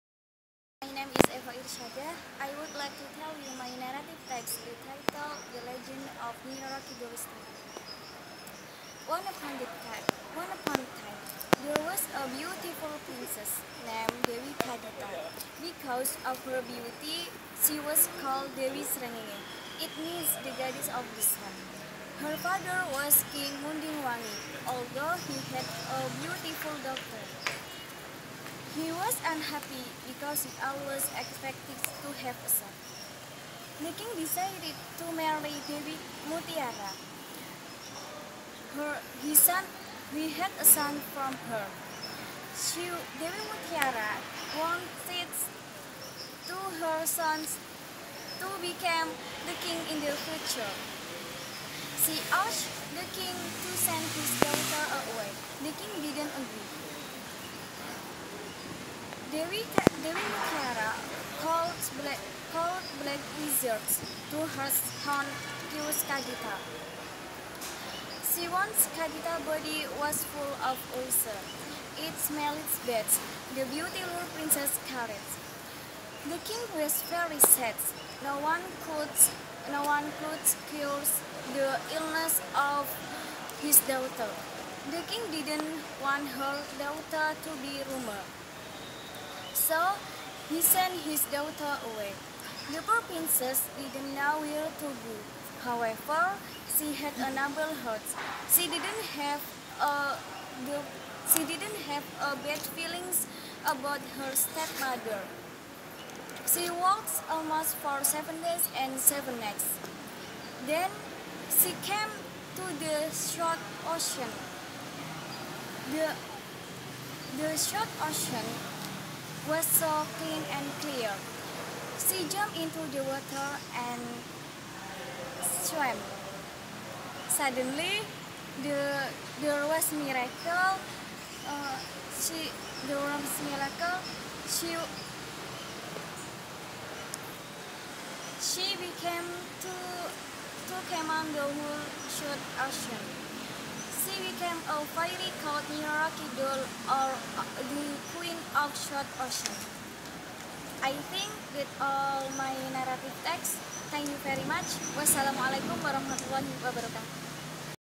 Eva-Nyi Roro Kidul
this is speaking 3 assigment with Ms. Rina (narrative text)